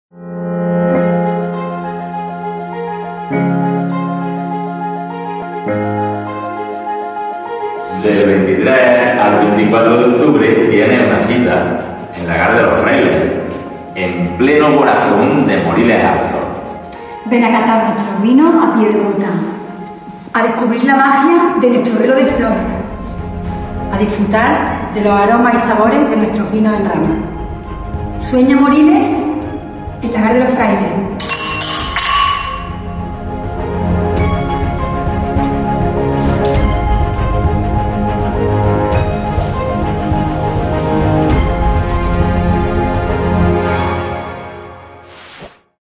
Lagar Los Frailes
Rodeado por 11 Has. de viñedo, en la actualidad el lagar mantiene su estructura arquitectónica original de un patio central de labor en torno al cual se agrupan varias bodegas, una sala de catas y la zona de envasado.